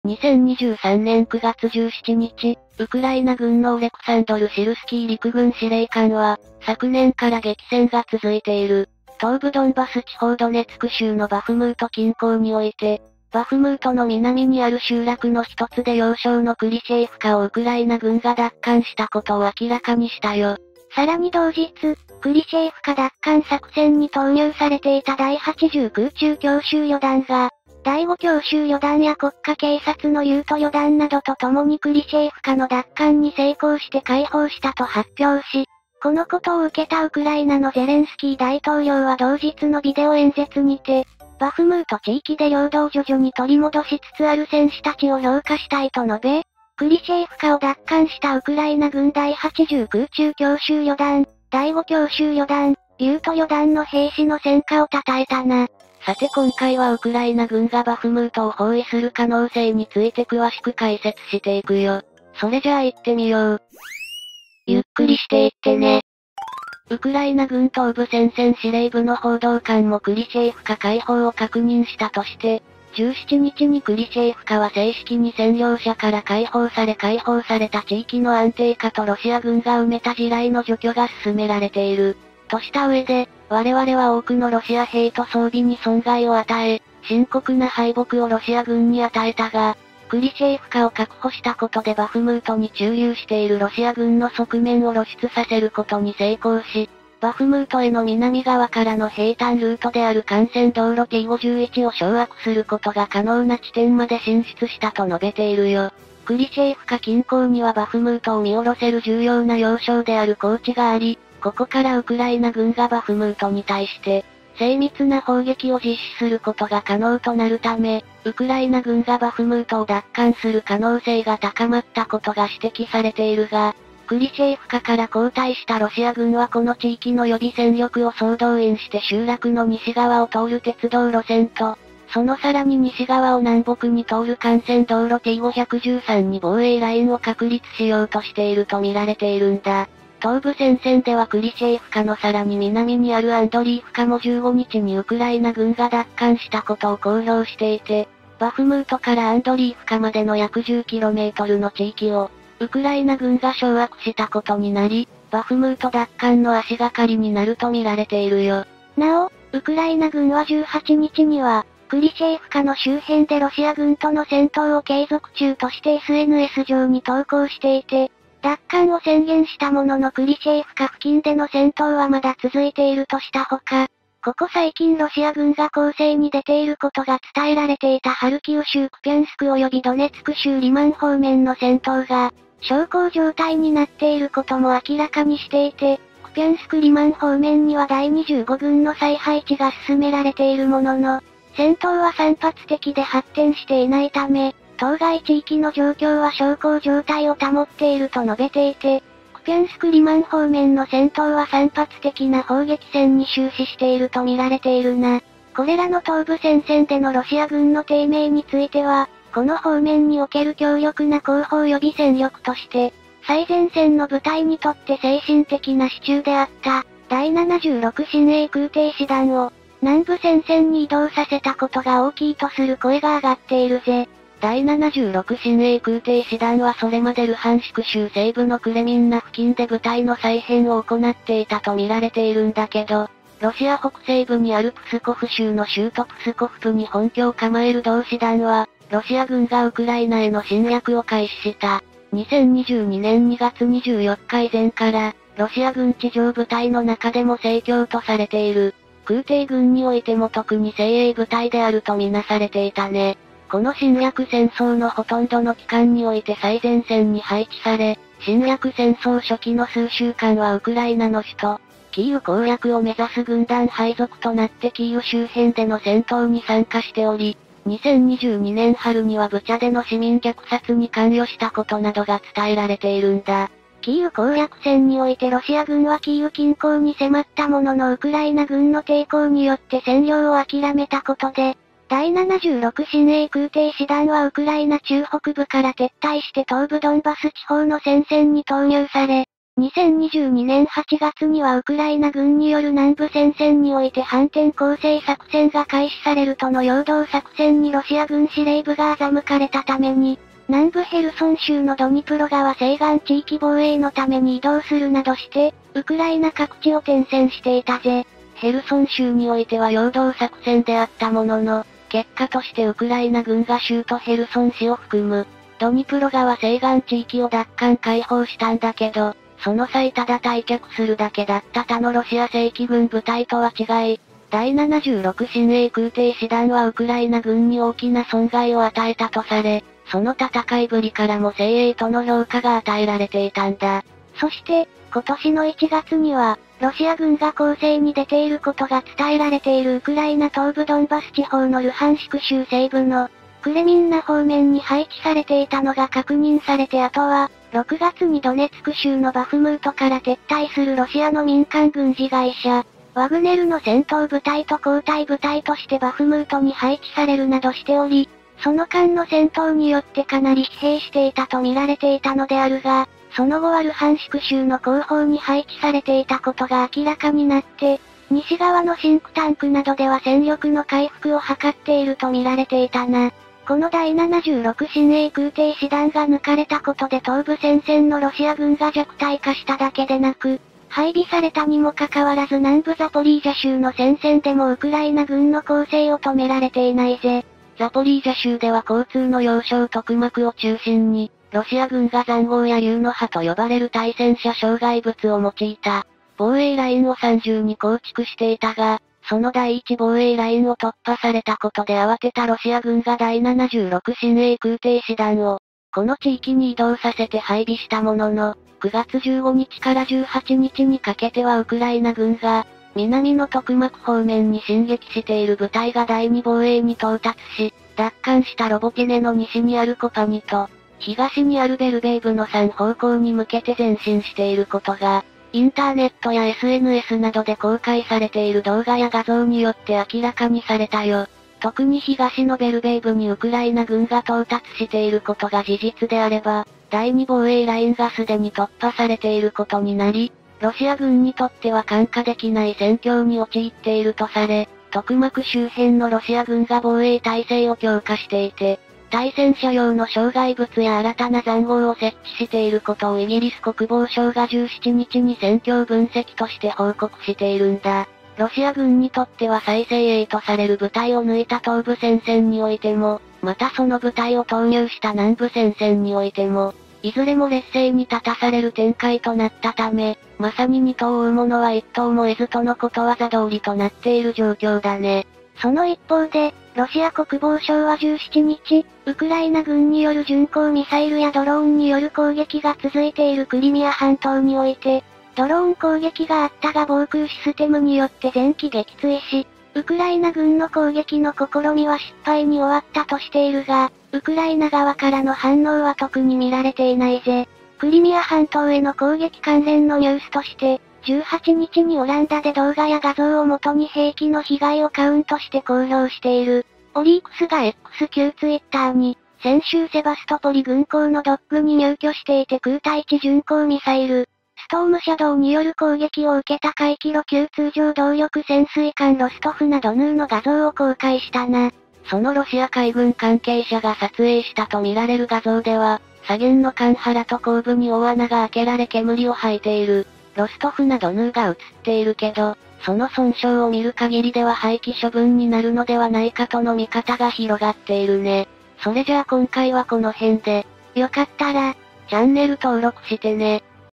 【ゆっくり解説・軍事News】クリミア半島奪還間近ザポリージャで露軍弾薬枯渇で悲鳴か！クリシェイフカ奪還で幹線道T0513進軍で露軍逃亡？
引用・転載「出典：海上自衛隊ホームページ」
引用・転載「出典：航空自衛隊ホームページ」
引用・転載「出典：陸上自衛隊ホームページ」